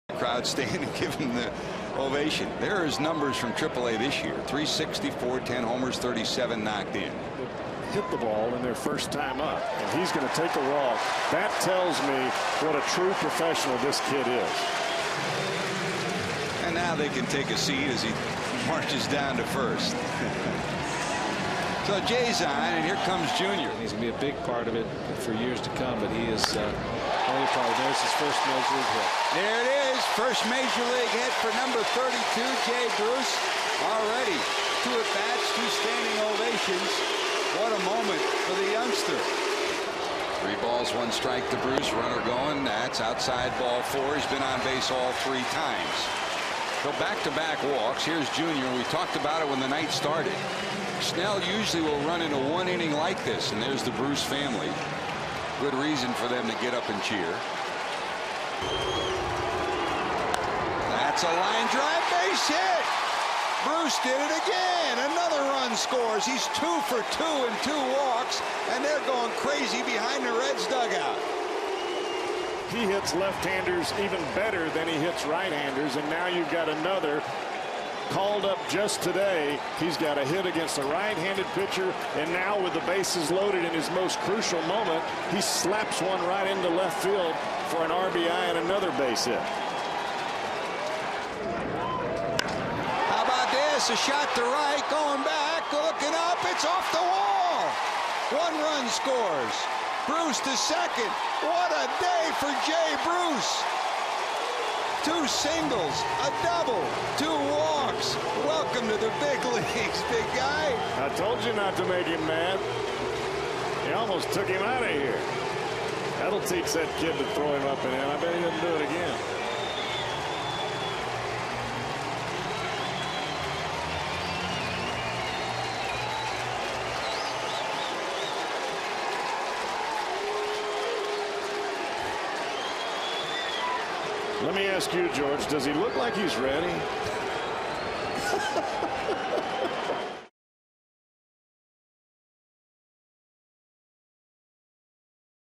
Jay Bruce goes 3-for-3 with two RBIs in his debut
5/27/08: In his big league debut for the Reds, Jay Bruce goes 3-for-3 with two RBIs and two walks